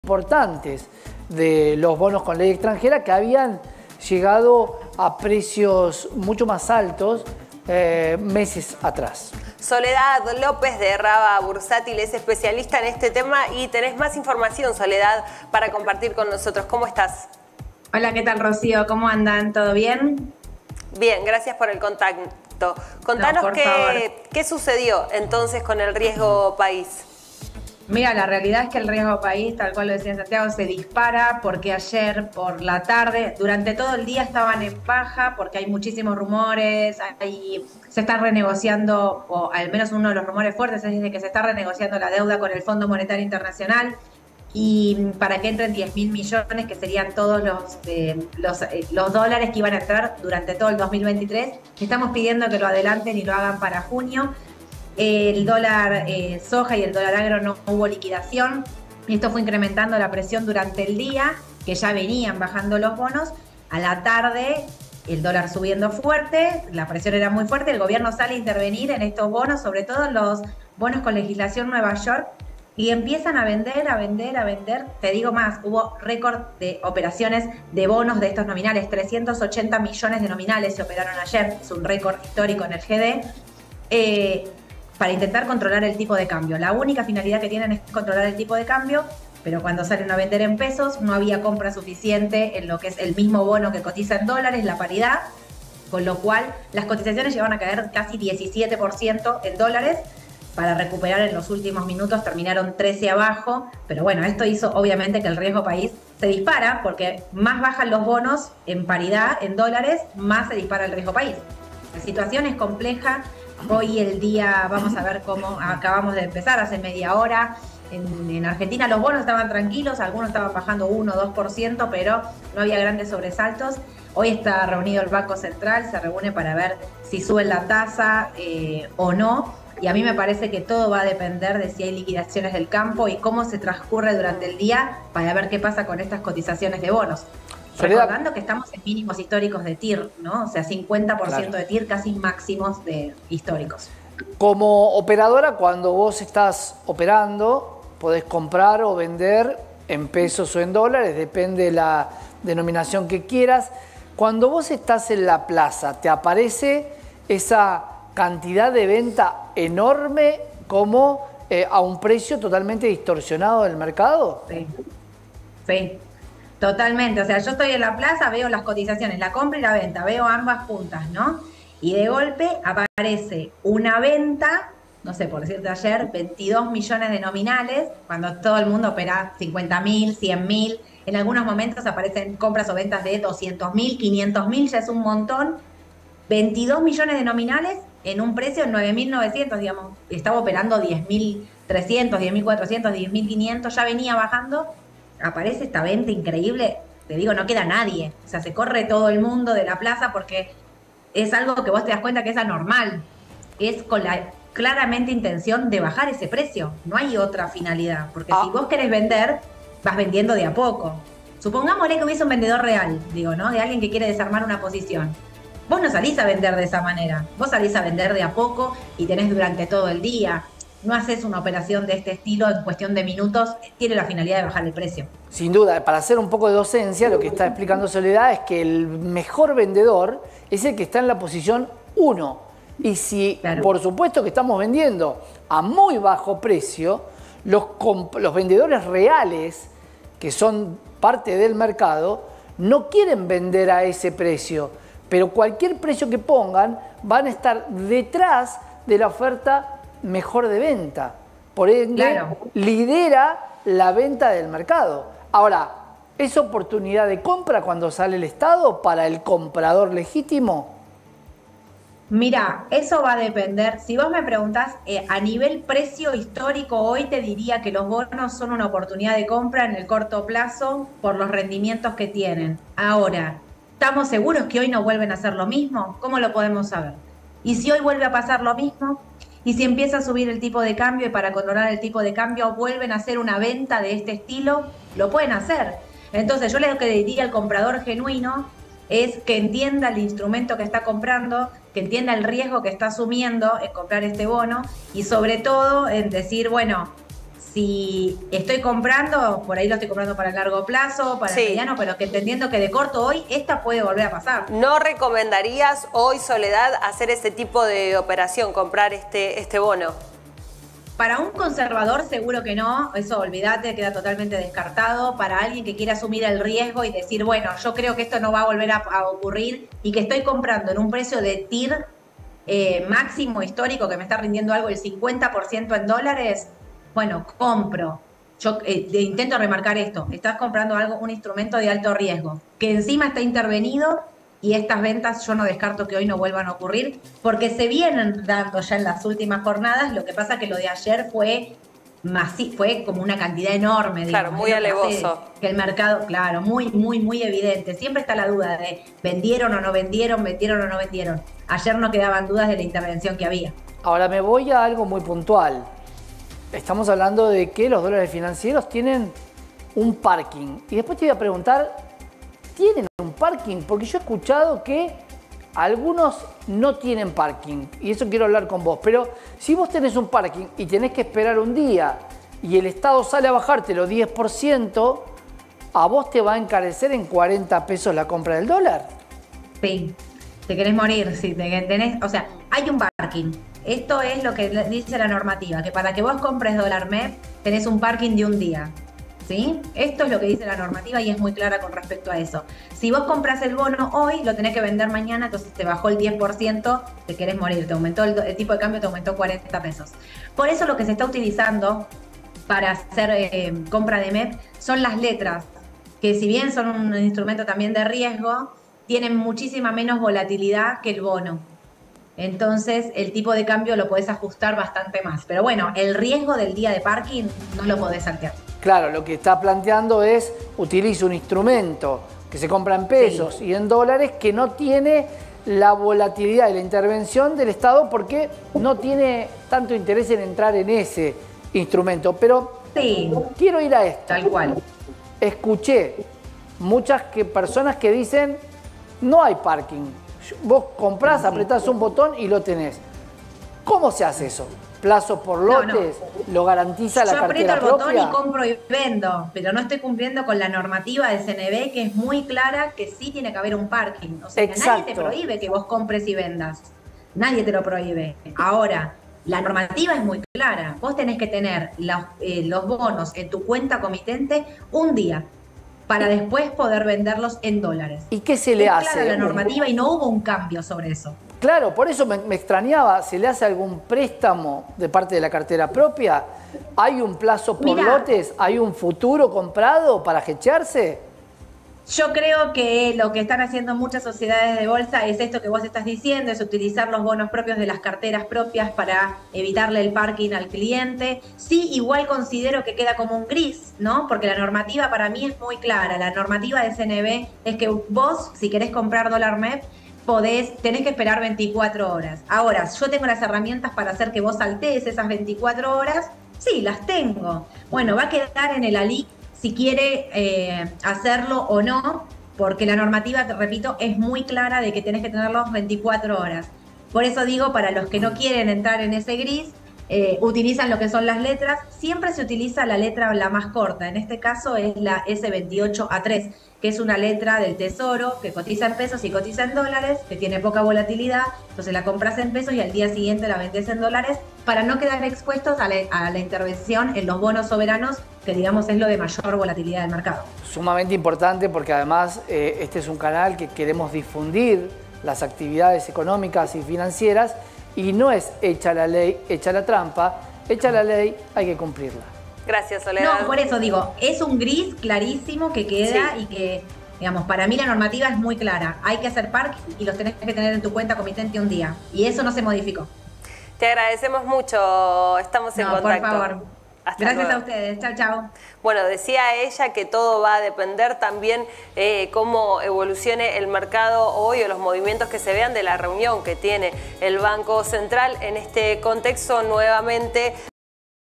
Bajan los bonos y se dispara el riesgo país (Canal E) 20/4
Entrevista a Soledad López en Canal E. Nuestra responsable de Desarrollo mencionó: “El riesgo país se disparó por los rumores de que se está renegociando la deuda con el FMI, no hubo liquidación del dólar soja y eso hizo que las reservas bajen fuerte”.